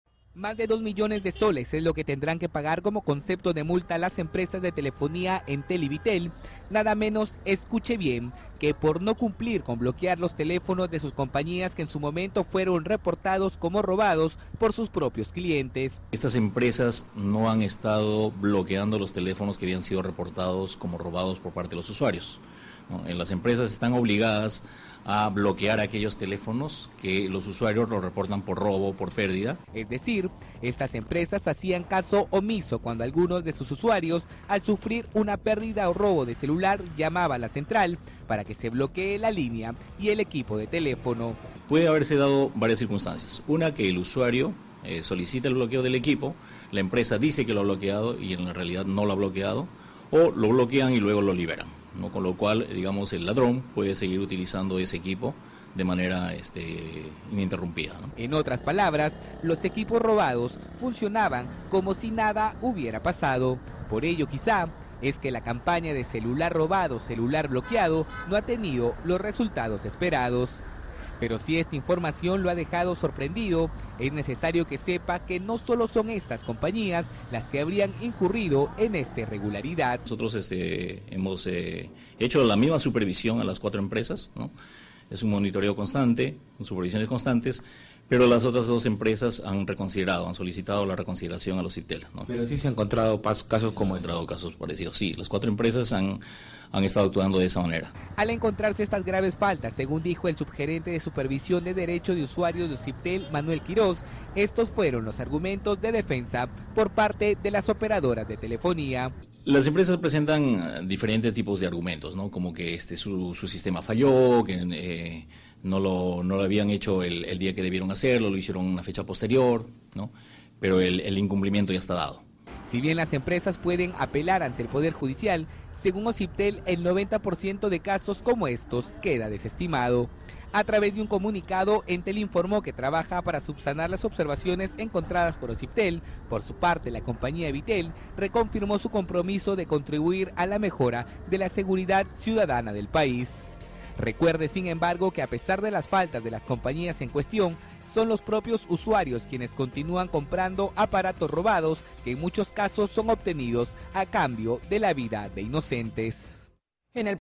Multan a Bitel y Entel por más de S/. 2 millones por prestar servicios a teléfonos robados
Osiptel confirmó multa de más de 2 millones de soles a las empresas de telefonía Entel y Bitel por no cumplir con bloquear a los teléfonos de sus compañías que en su momento fueron reportados como robados por sus clientes.

Según Manuel Quiroz Muñoz, subgerente de supervisión de Derecho de Usuarios de Osiptel, indicó que estas empresas no habían estado bloqueando los equipos que habían sido reportados como robados y “”las empresas están obligadas a bloquear aquellos teléfonos que los usuarios reportan por robo o pérdida”, explicó.

Quiroz Muñoz también mencionó que las operadoras de telefonía dieron diferentes explicaciones como que su sistema falló, que no lo hicieron el día que debieron hacerlo, pero lo cierto es que el incumplimiento ya está dado.

Las empresas pueden apelar ante el Poder Judicial, pero según Osiptel el 90% de estos casos queda desestimados. A través de un comunicado, Entel informó que trabaja para subsanar las observaciones encontradas por Opsitel. Mientras, Bitel reconfirmó su compromiso de contribuir a la mejora de la seguridad ciudadana del país.